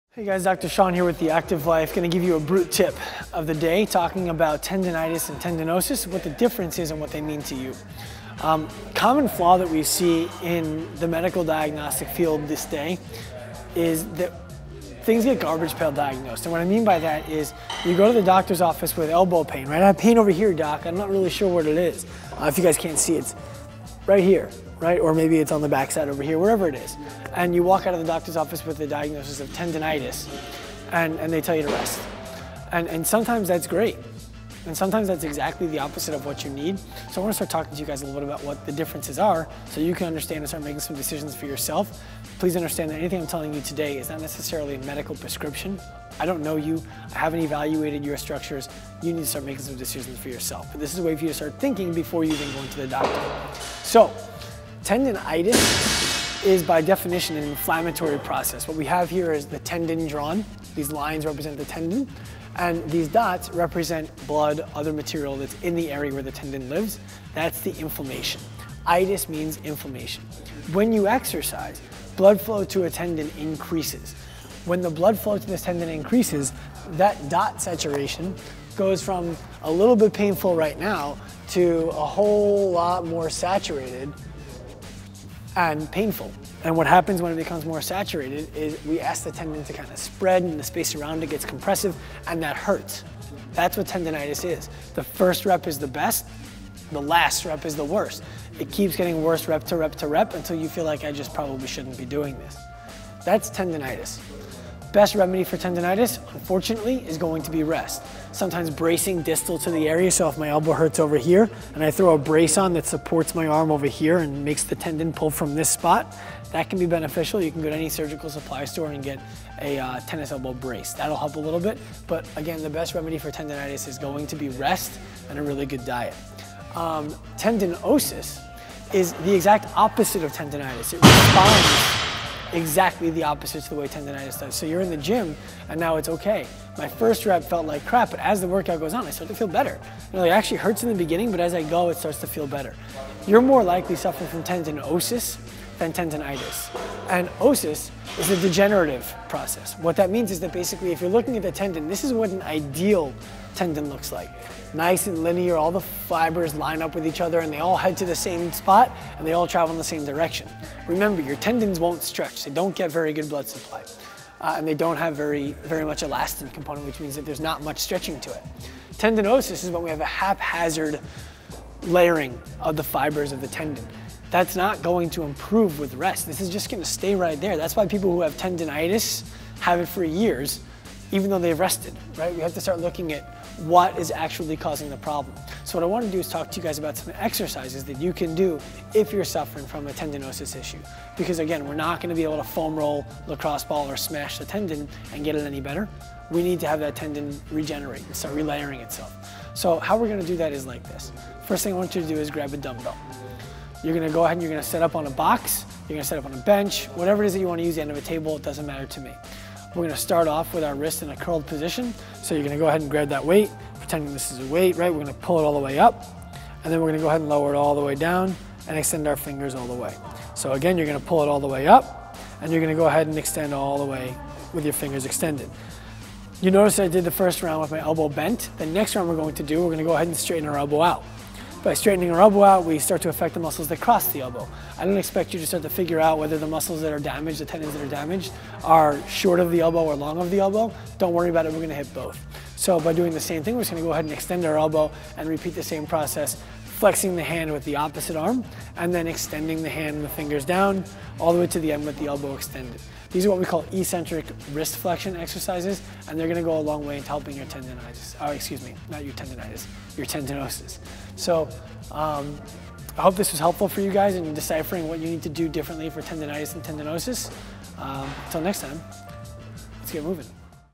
Tenditis v. Tendinosis
Often times athletes will visit a doctor because they are dealing with chronic or acute pain and they are concerned about being able to continue exercising. The diagnosis is overwhelmingly "tendinitis" and the prescription is "rest". While rest would be the perfect prescription for an athlete with tendinitis, there is a large group of people who walk out of their doctors office with a handful of prescriptions and a mandate of rest who don't need either. If you are suffering from tendinosis (not the same as tendinitis), your needs are not only different, but they are the opposite. Watch this video to see if your prescription has been fitting your diagnosis.